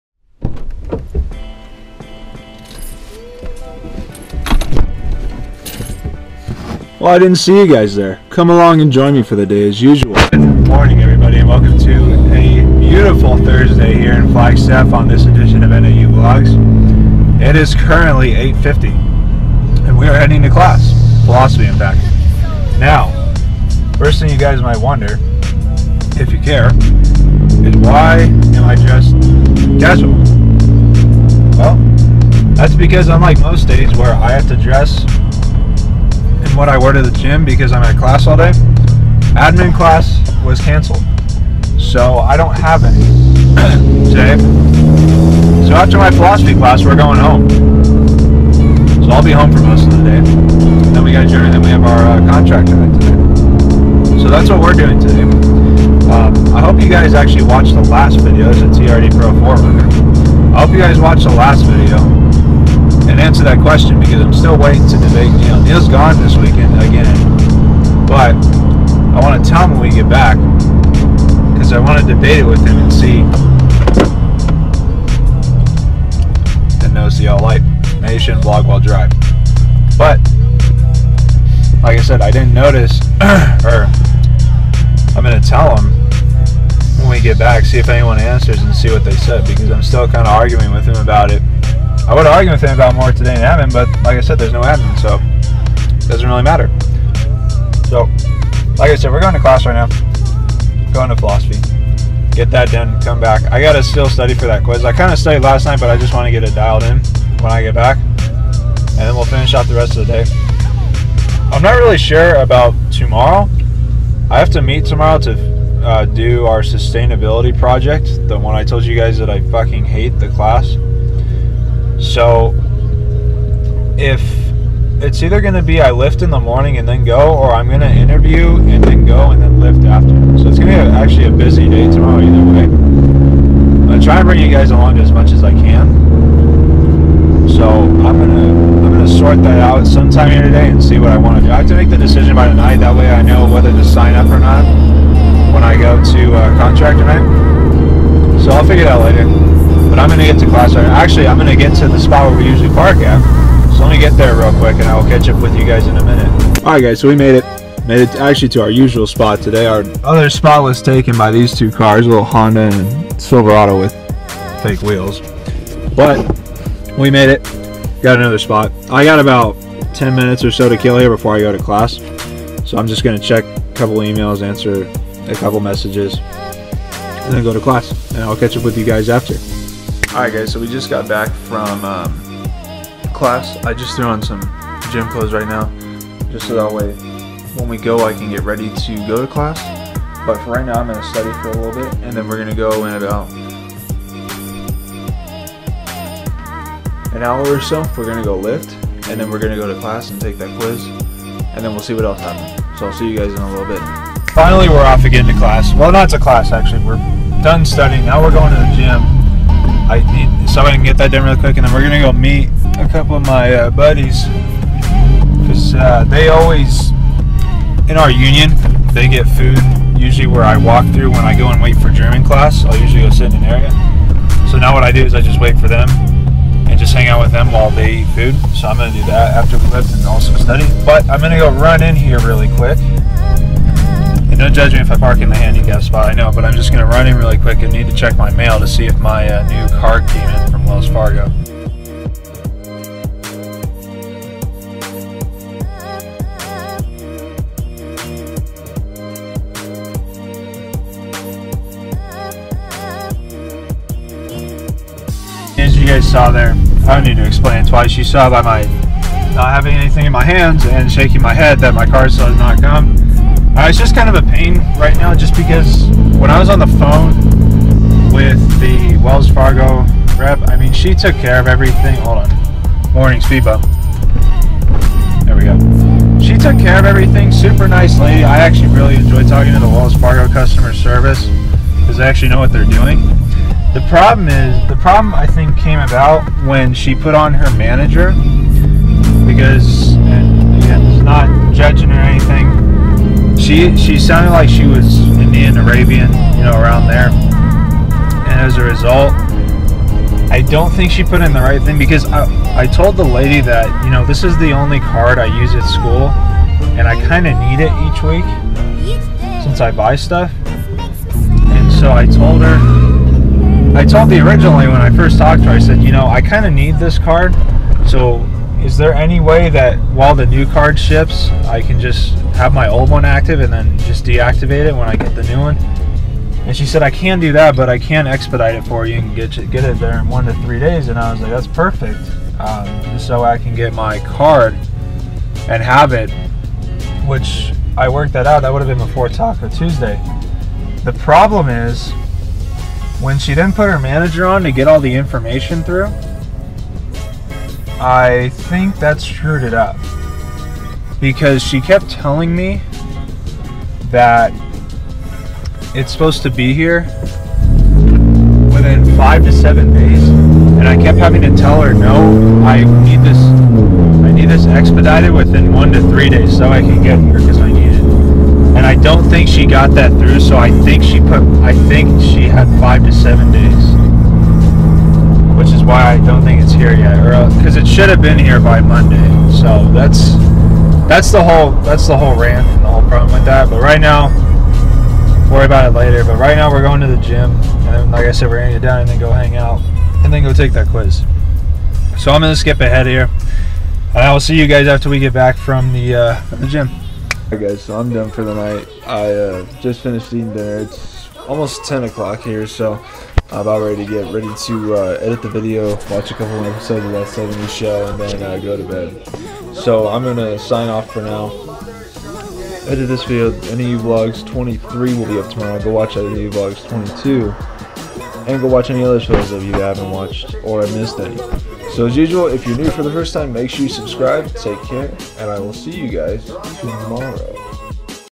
NAU VLOGS #24....Cancelled Class, Gym, and Mail Check!
Cancelled class days are the best days!